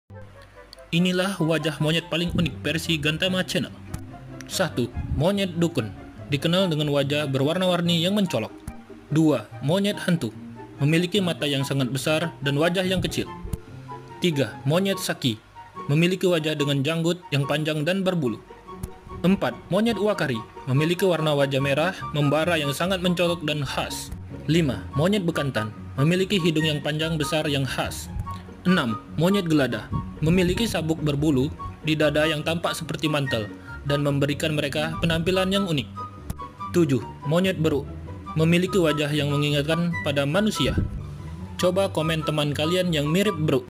7 wajah monyet yang mirip dengan manusia jenis monyet
Inilah wajah monyet paling unik versi gantama chanel

1. Monyet dukun/mandrill
Monyet mandrill dikenal dengan wajah berwarna-warni yang mencolok

2. Monyet hantu/tarsius
memiliki mata yang sangat besar dan wajah yang kecil

3. Monyet saki
memiliki wajah dengan janggut yang panjang dan berbulu

4. Monyet uakari
memiliki warna wajah merah membara yang sangat mencolok dan khas

5. Monyet bekantan
memiliki hidung yang panjang besar yang khas

6. Monyet gelada
memiliki "sabuk berbulu" di dada yang tampak seperti mantel dan memberikan mereka penampilan unik

7. Monyet beruk
memiliki wajah yang mengingatkan pada manusia